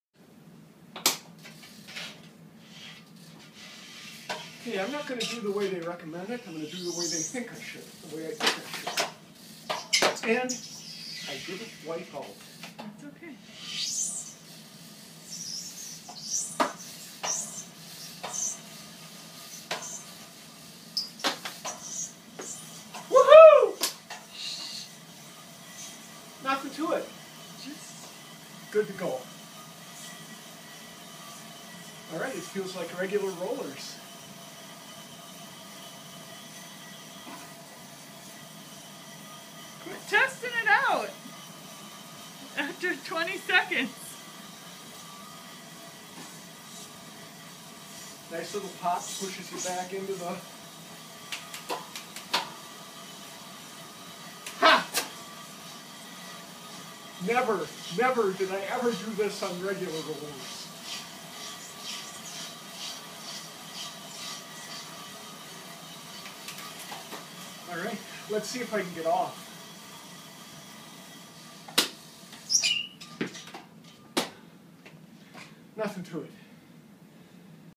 E-Motion Rollers. First Ride
I have many hours in conventional rollers. Those rollers are much more stable and much, much easier to ride!!!